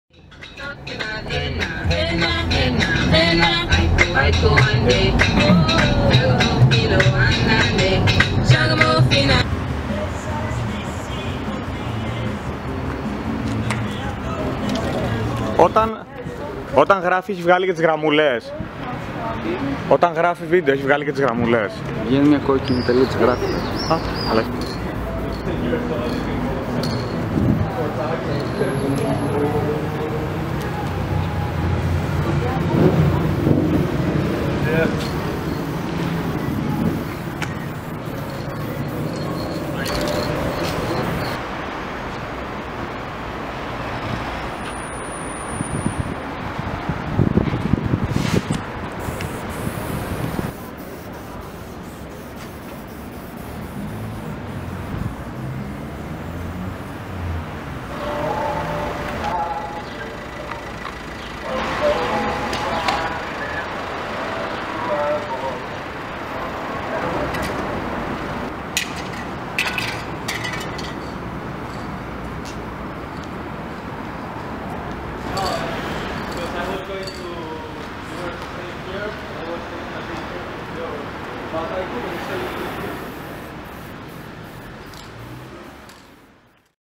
SWITCH & HAVE YOU FILLED YOUR VOID
Graffiti day with my friend and artist Exit at Pireus port